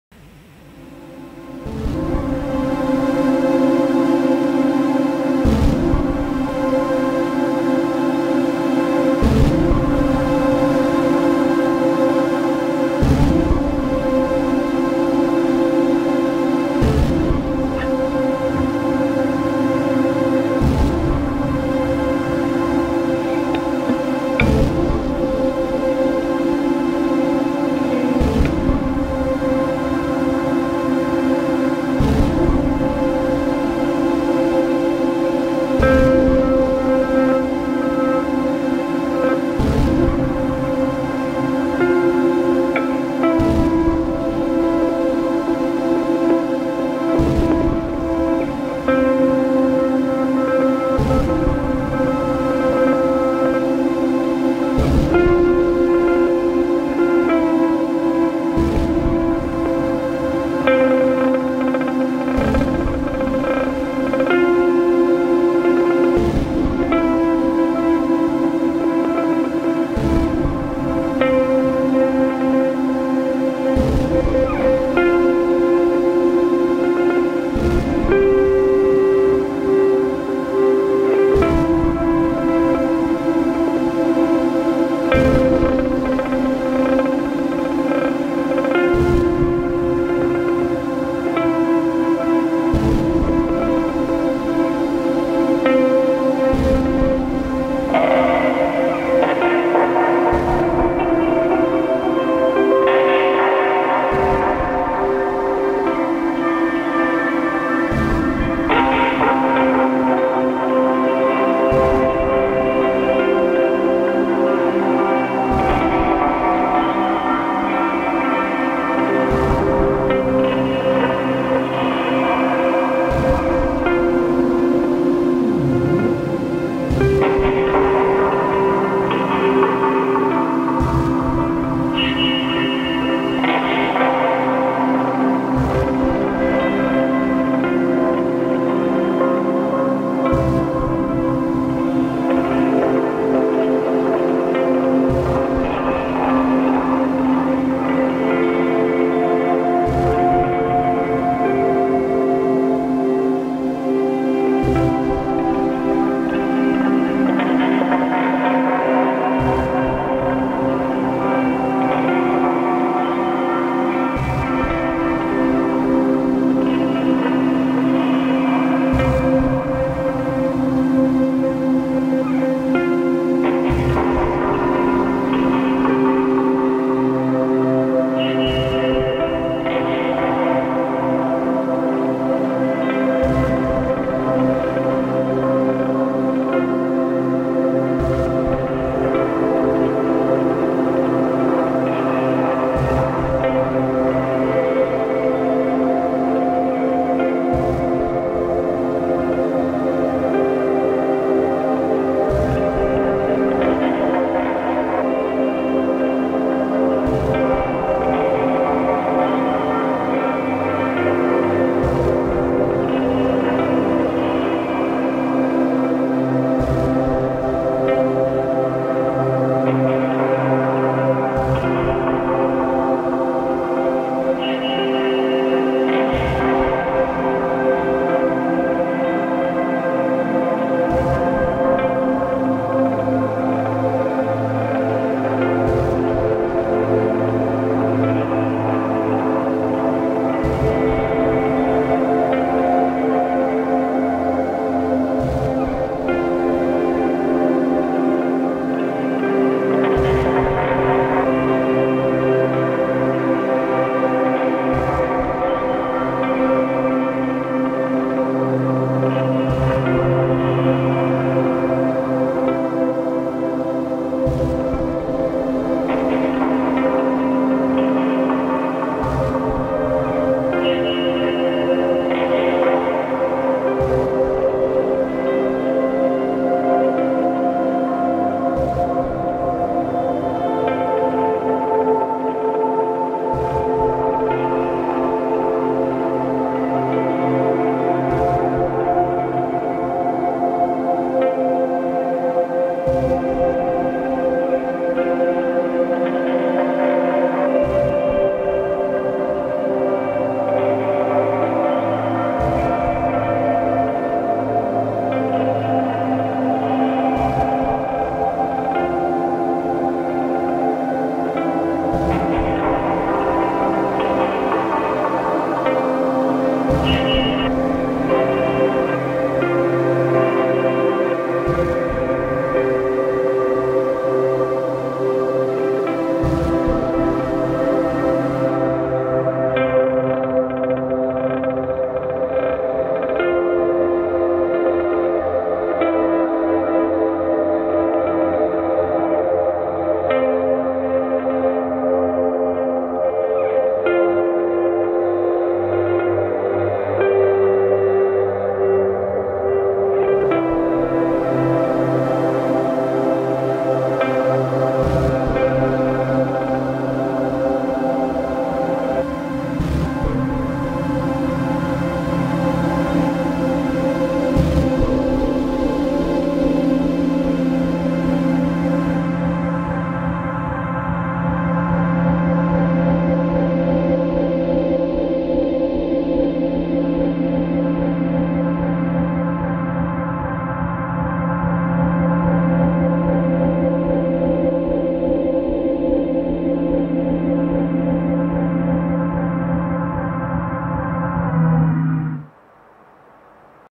Lyra8 & guitar
Got myself an early birthday present. The lyra8 fells like a drunken orchestra under the sea. Still don't know how to conduct it, but it works great with some guitar swells and a bow.